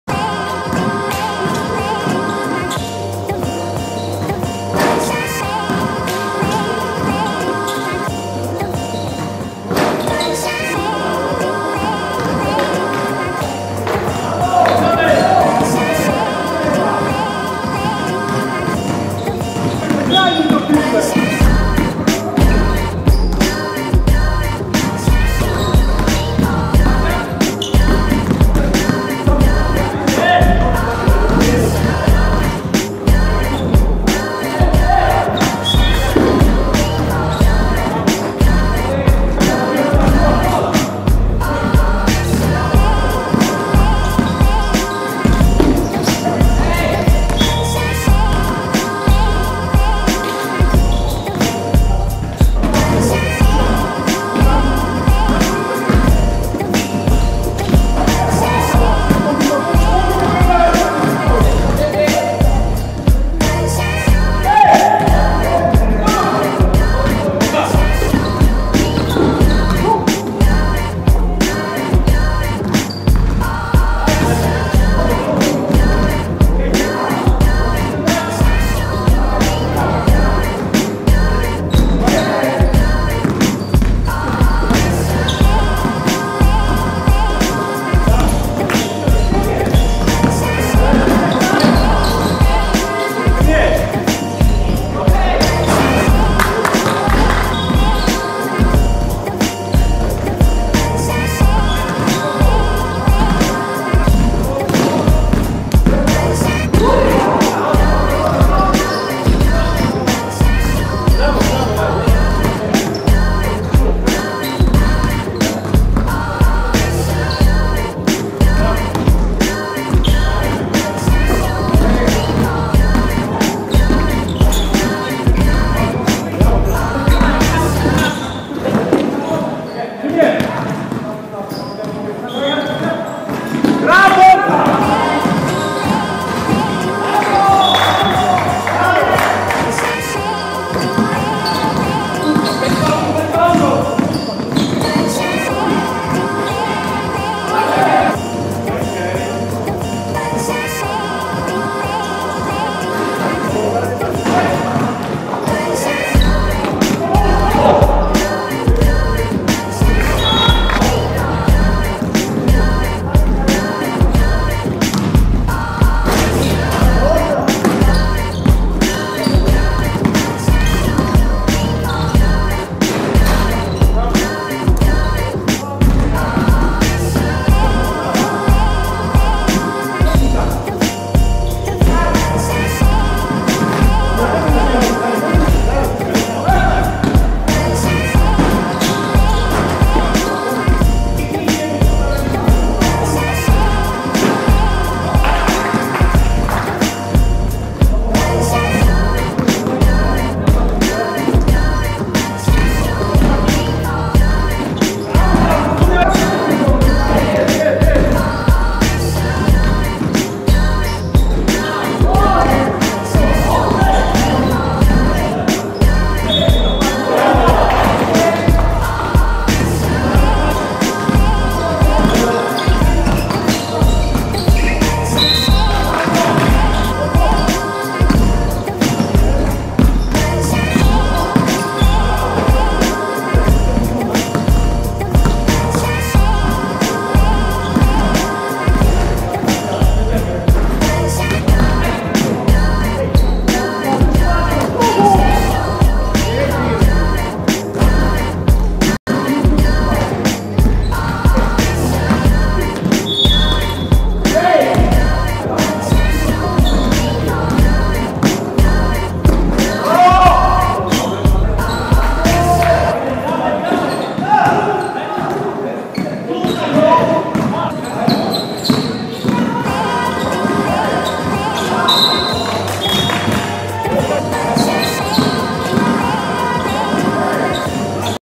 24 11 I Runda San Antonio vs New York – Basket 36+
SAN ANTONIO 55:59 NEW YORK
P.Kluźniak 18pkt 29zb 6ast
P.Smolaga 26pkt 12zb 6ast 3stl